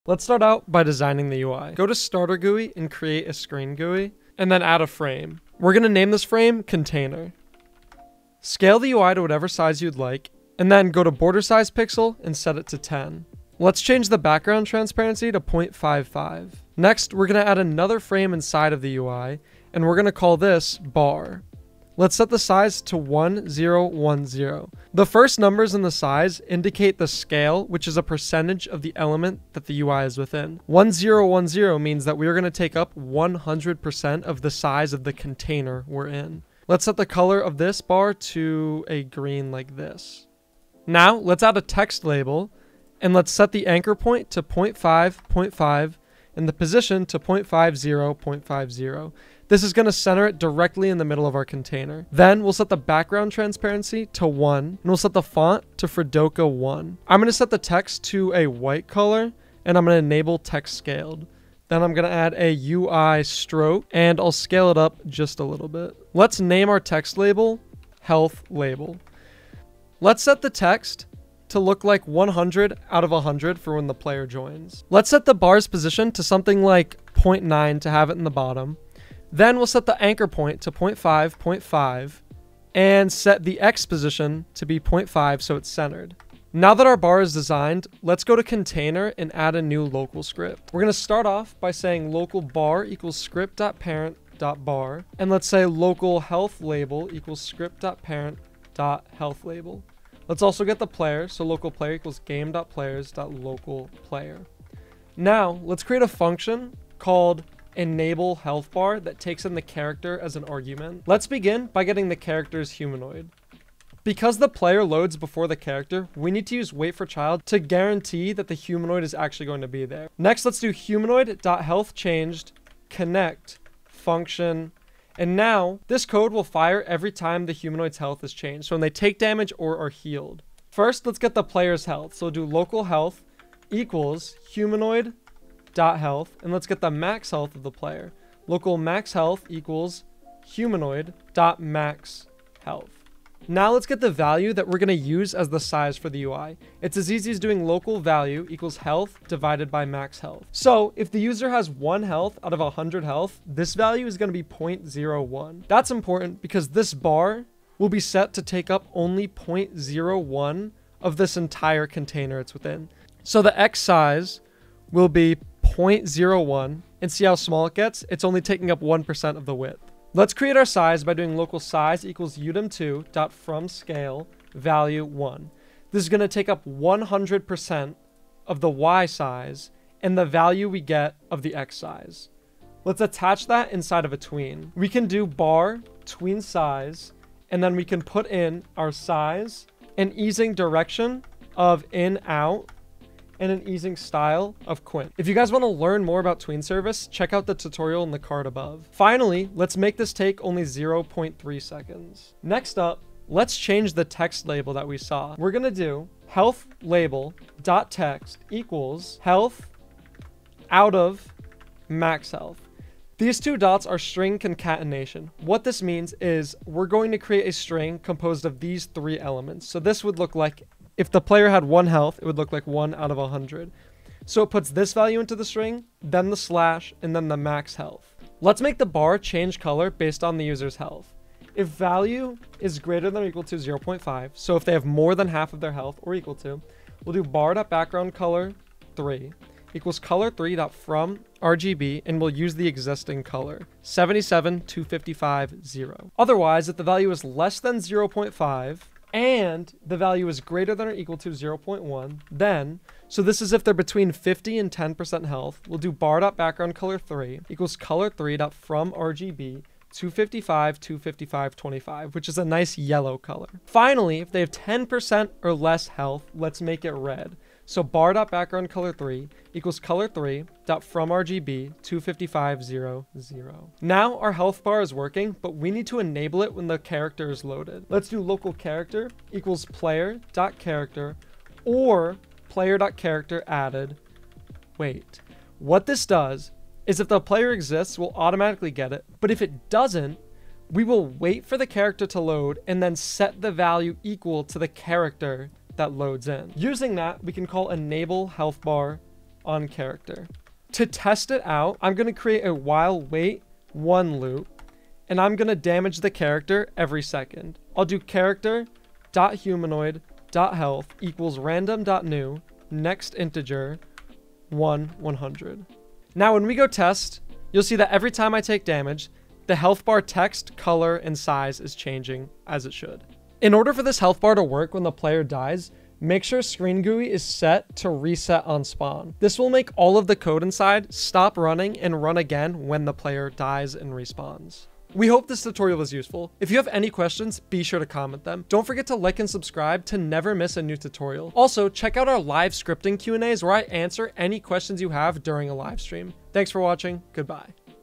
How to Make a HEALTH BAR in 5 MINUTES | Roblox Beginner Scripting Tutorial (2023)
This easy Roblox scripting tutorial for new Roblox developers teaches you how to make an easy health bar system. This is super popular for Roblox RPG games and FPS games. Games like Phantom Forces, Bad Business, World Zero, and more use custom health bars in their game. If you're a beginner Roblox scripter, this tutorial will be easy for you to learn to script.

TIMESTAMPS:
00:00 - 01:24 Designing the UI
01:25 - 05:00 Scripting the UI
05:00 - 05:47 Testing & Conclusion